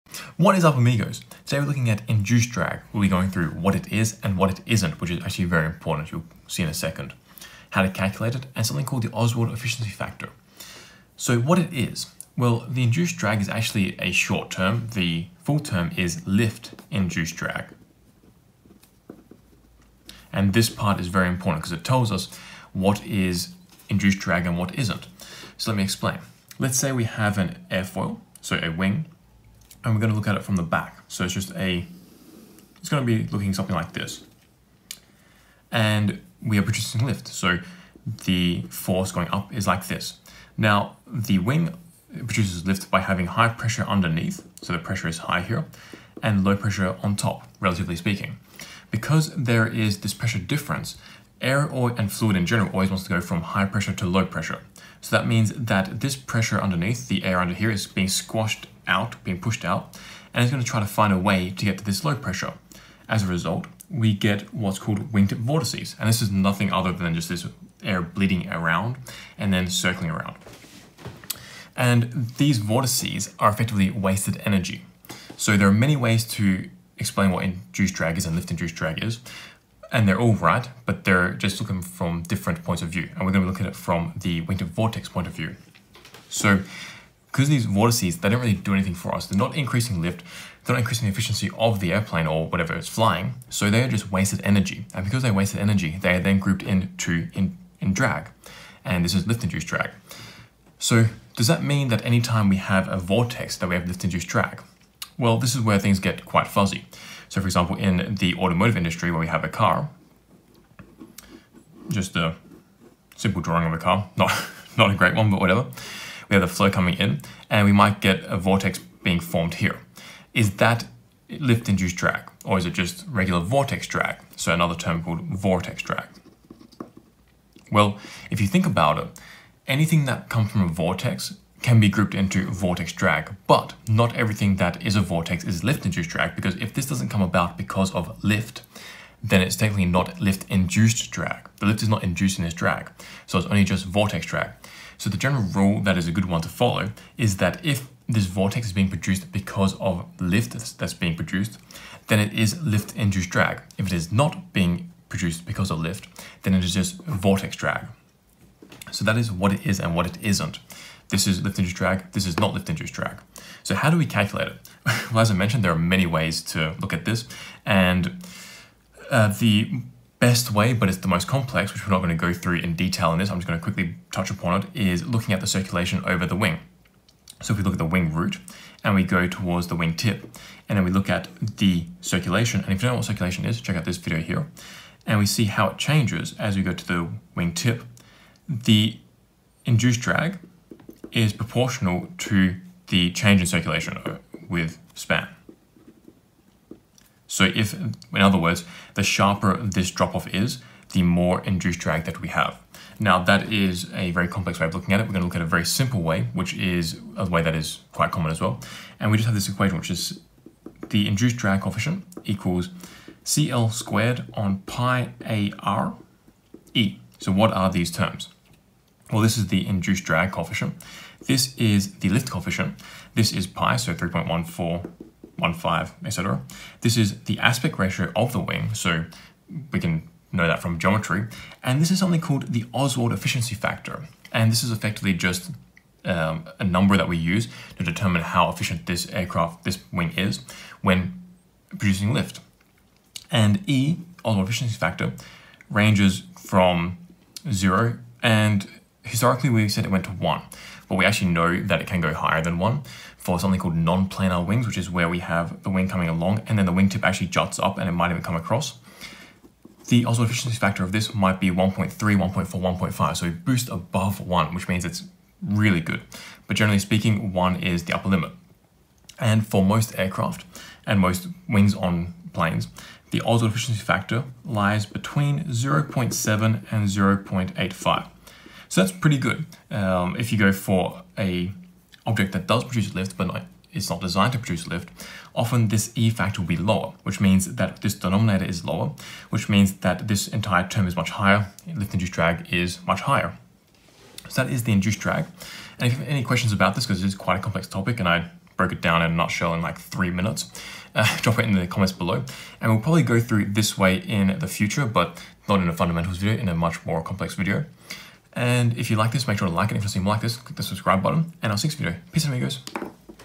[Aero Fundamentals #30] Induced Drag Explained
Induced drag (Lift Induced Drag) is one of the most important components of drag. But what is it exactly? And what isn't it? How is it created? How is it different to vortex drag? How do you calculate it? And what is the Oswald Efficiency Factor?

That's what we cover in this video.

Want to get better at CFD? Take our CFD course